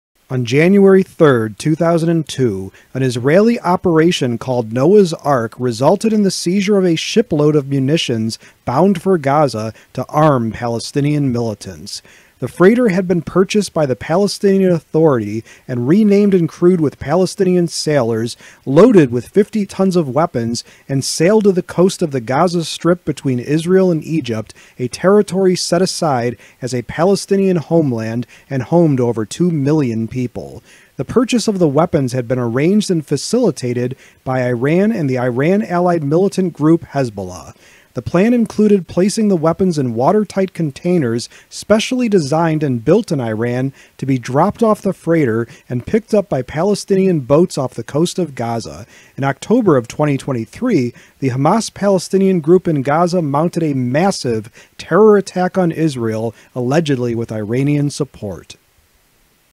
History Short: Israel Seizes Munitions Ship Bound for Gaza
On January 3, 2002, an Israeli operation called “Noah’s Ark” resulted in the seizure of a ship load of munitions bound for Gaza to arm Palestinian militants.

The text narrated in this video is from the original article

The images by Avishai Teicher via the PikiWiki - Israel free image collection project available by David Memi via the PikiWiki - Israel free image collection project available and by Yehudit Garinkol via the PikiWiki - Israel free image collection project available are licensed under the Creative Commons Attribution 2.5 Generic license